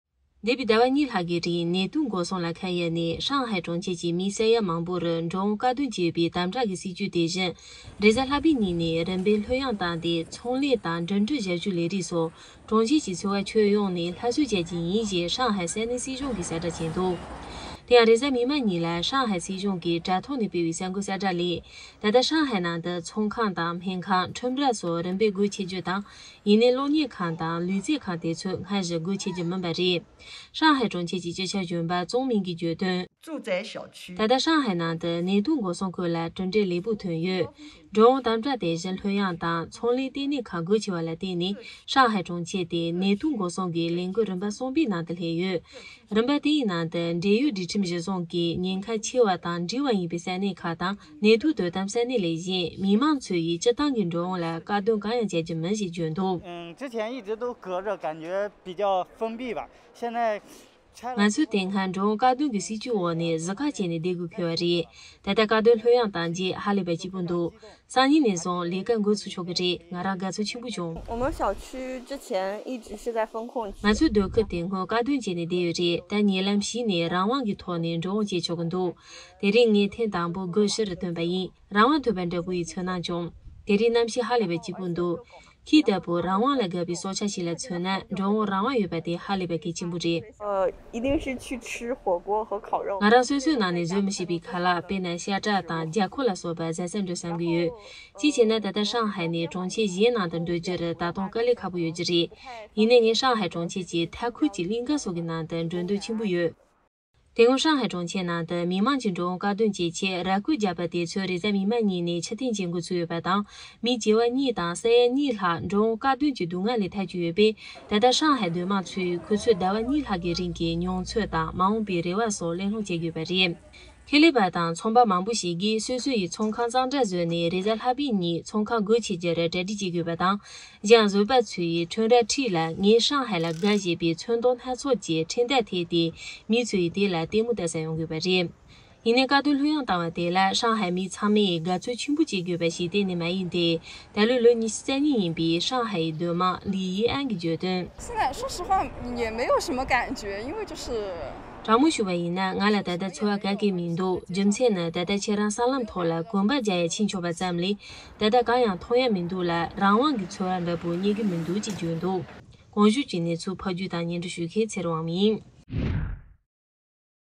COVID-China Shanghai ཧྲང་ཧེར་ནད་འགོག་དམ་དྲག་རིམ་གྱིས་ཕྱིར་འཐེན།
འདས་པའི་ཟླ་བ་གཉིས་ལྷག་གི་རིང་རྒྱ་ནག་གི་ཧྲང་ཧེ་གྲོང་ཁྱེར་ལ་ནད་དུག་འགོག་སྲུང་ལ་ཁ་གཡར་ནས་མི་ས་ཡ་མང་པོར་འགྲོ་འོང་བཀག་སྡོམ་བྱས་ཡོད་པའི་དམ་དྲག་གི་སྲིད་ཇུས་དེ་རེས་གཟའ་ལྷག་པའི་ཉིན་ནས་རིམ་བཞིན་ལྷོད་ཡངས་བཏང་སྟེ་ཚོང་ལས་དང་འགྲིམ་འགྲུལ་ཞབས་ཞུའི་ལས་རིགས་སོགས་གྲོང་ཁྱེར་གྱི་འཚོ་བ་ཁྱོན་ཡོངས་ནས་སླར་གསོ་བྱ་རྒྱུ་ཡིན་ཞེས་ཧྲང་ཧེ་ས་གནས་སྲིད་གཞུང་གིས་གསལ་བསྒྲགས་སྤེལ་འདུག